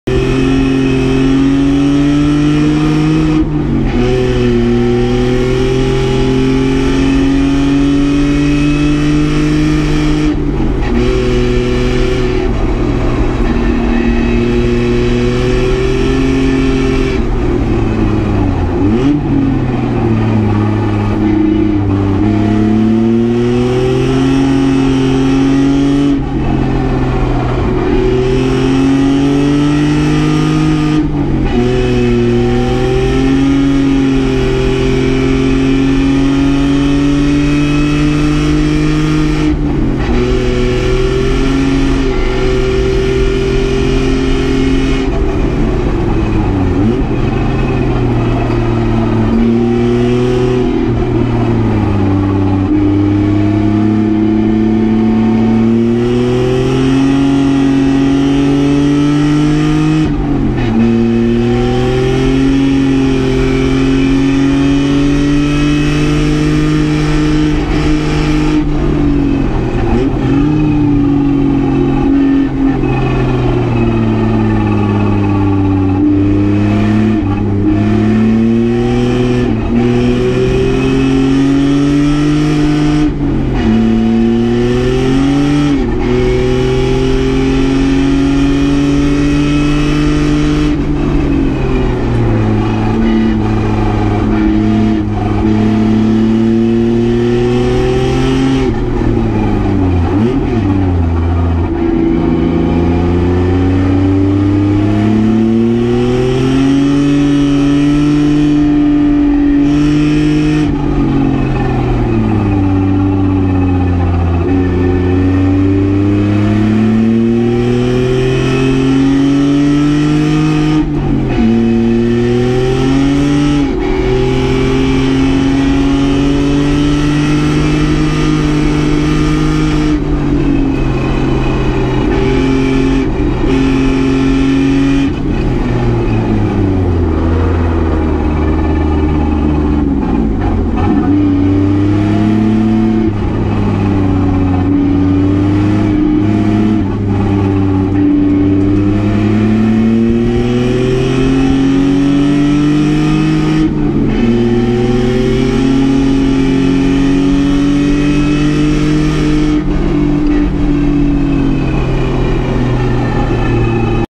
Calabogie team fastest lap (ChumpCar)
Event: ChumpCar (Sept 2017)
Track: Calabogie Motorsport Park
Driver: Anton
Description: Our team's fastest lap 2:29.762